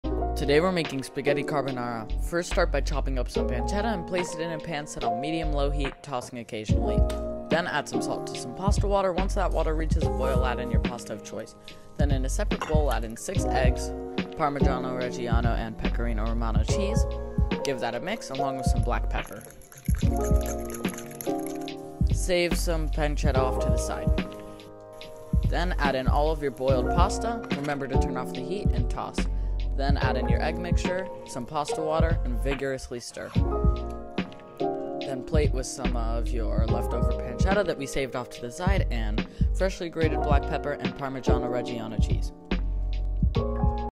Spaghetti Carbonara Traditionally.
Hello, I hope you enjoy this quick on how to make spaghetti carbonara.